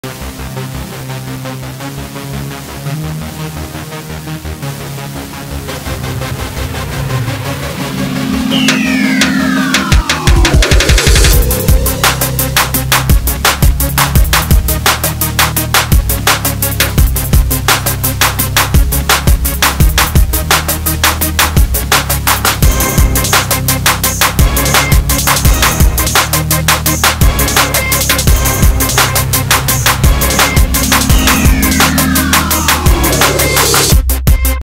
Pitbull climbing on the Tree
Oscar from Greece
Slow motion jumping